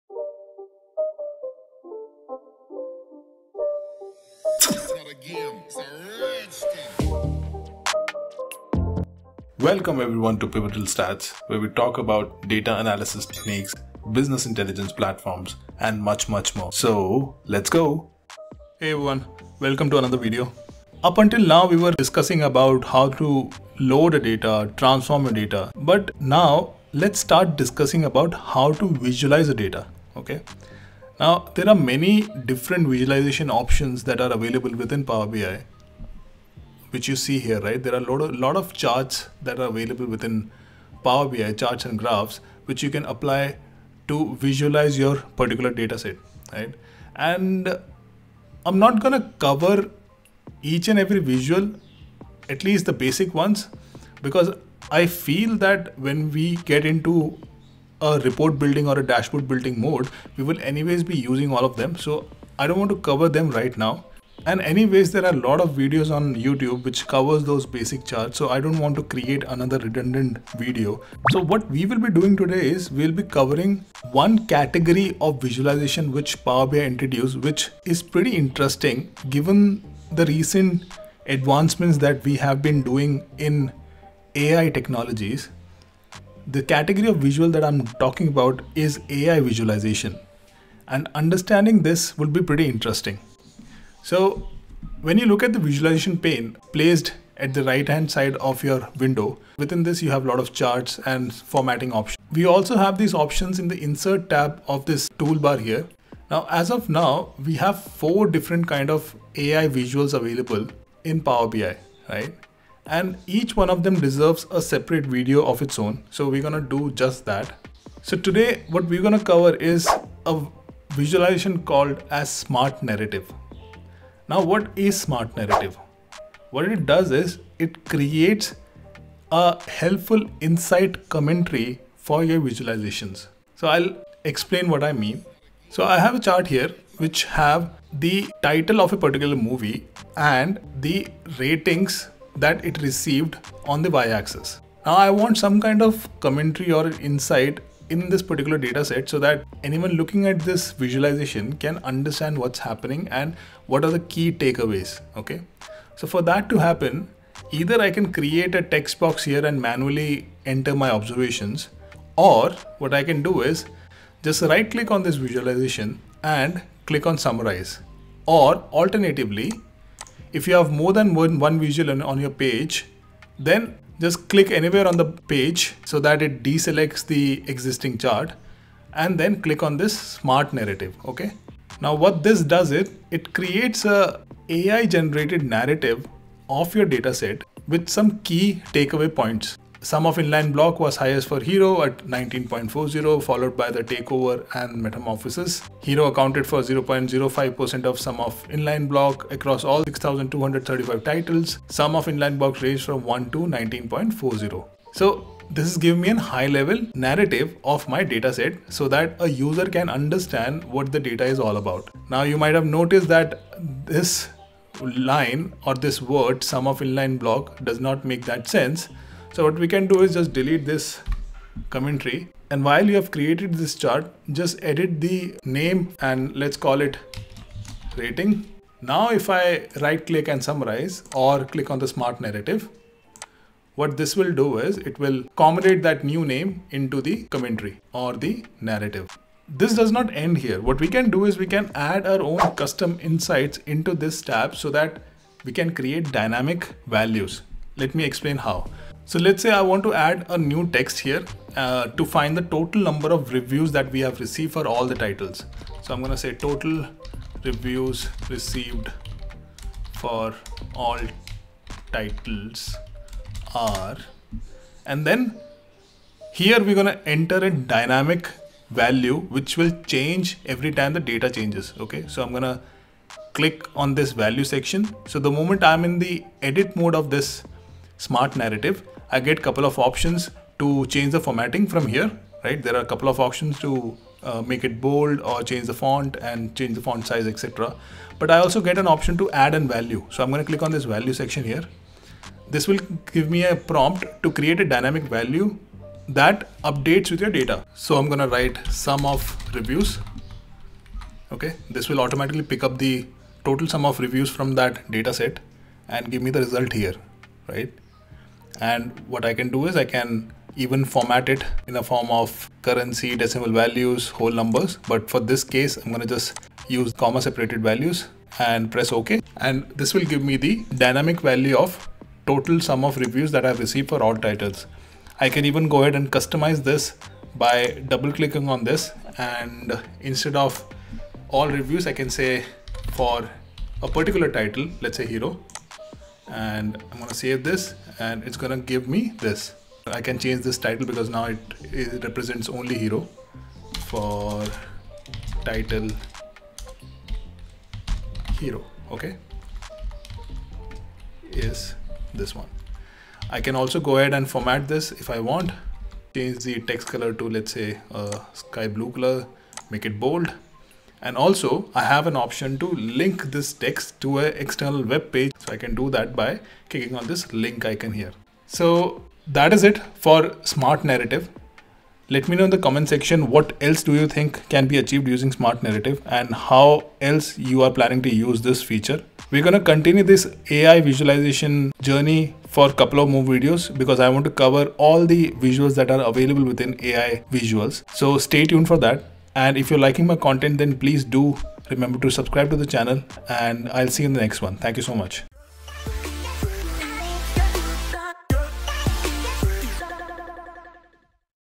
AI Visualization - Generate Automatic Insights & Narrative using SMART NARRATIVE visual | POWER BI
AI Visuals of Power BI are a powerful set of tools to quickly increase your productivity & utilize the power of artificial intelligence in your reports. This is the first video of the AI visual series where we will understand how to work with SMART NARRATIVE

1. Intro to SMART NARRATIVE
2. How to implement the visual
3. Creating dynamic Custom Defined Metric within the visual

1. Blue Yeti Microphone -

2. LG Ultrawide Curved Monitor -